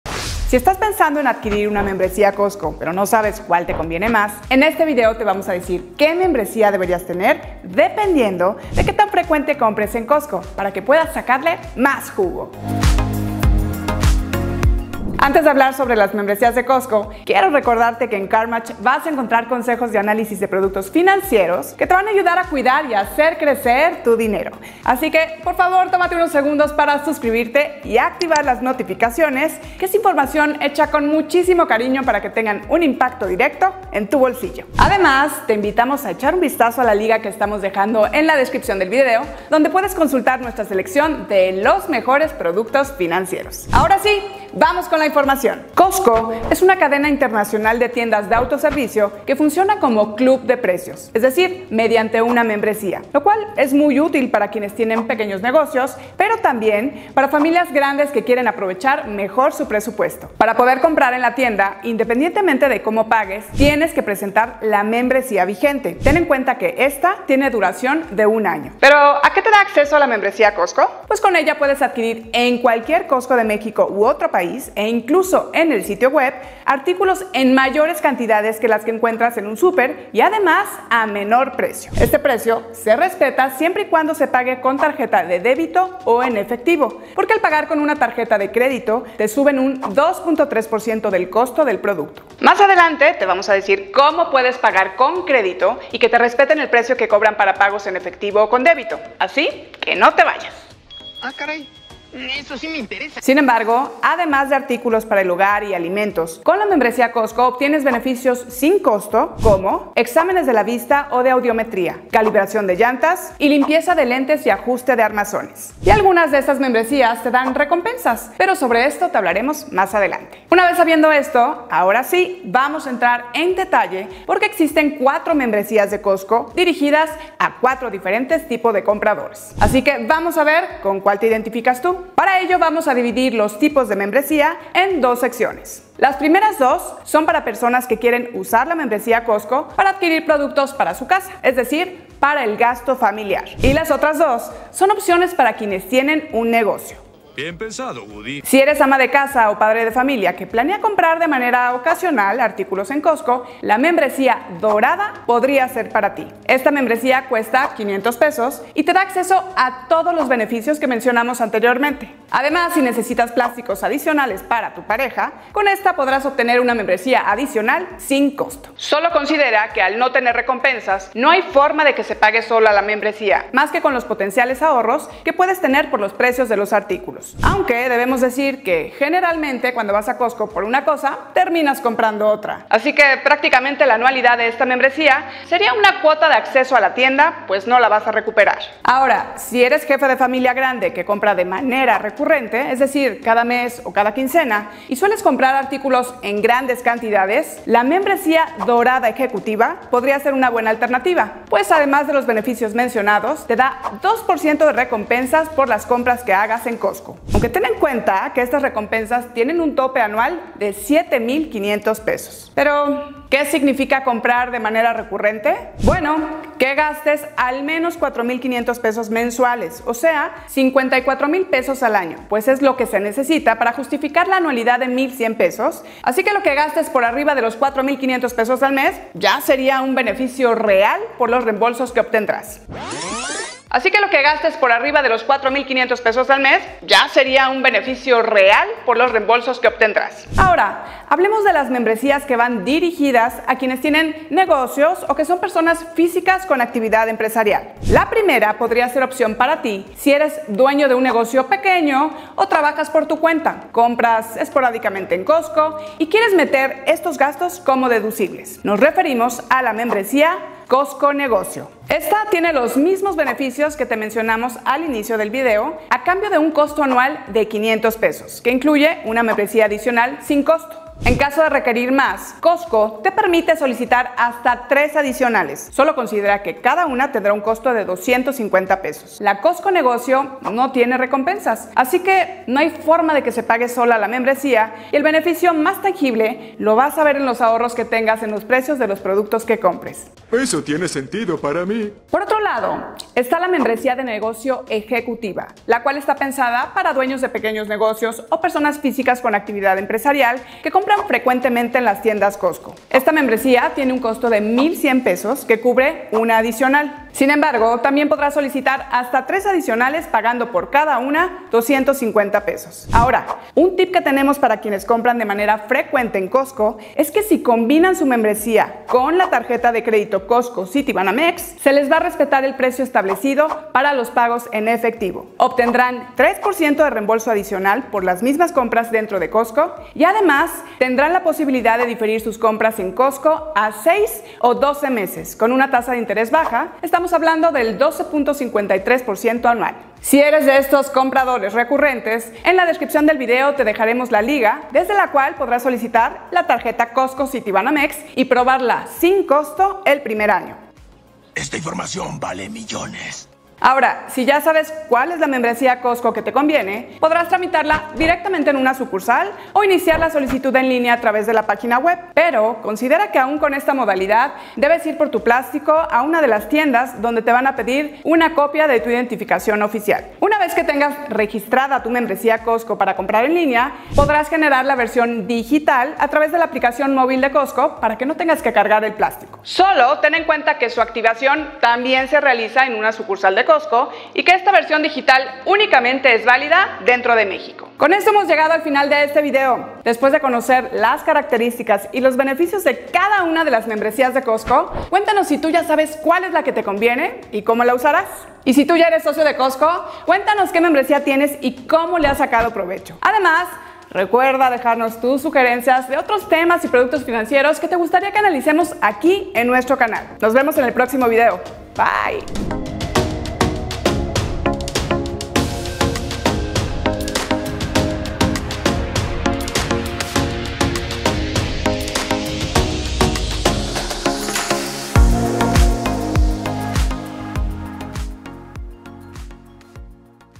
Membresías Costco: Precio, cuál te conviene solicitar y cómo tramitarlas
Si quieres saber el precio de cada membresía Costco, en este video te decimos sus costos, cuál de las 4 membresías Costco te conviene tramitar y cómo es el proceso de solicitud.

00:00 Introducción
01:08 Beneficios de la membresía Costco
02:00 ¿Qué tipos de membresías Costco existen?
02:28 ¿Cuál de las membresías Costco conviene para compradores ocasionales?
03:08 ¿Cuál de las membresías Costco conviene para compradores recurrentes?
04:00 ¿Cuál de las membresías Costco conviene para dueños de negocio que compran de manera esporádica?
04:52 ¿Cuál de las membresías Costco conviene para dueños de negocio que compran de manera frecuente?
05:12 ¿Cómo obtener hasta 5% de Cashback por compras en Costo?
05:53 ¿Cómo sacar la membresía Costco? ¿Se puede hacer en línea?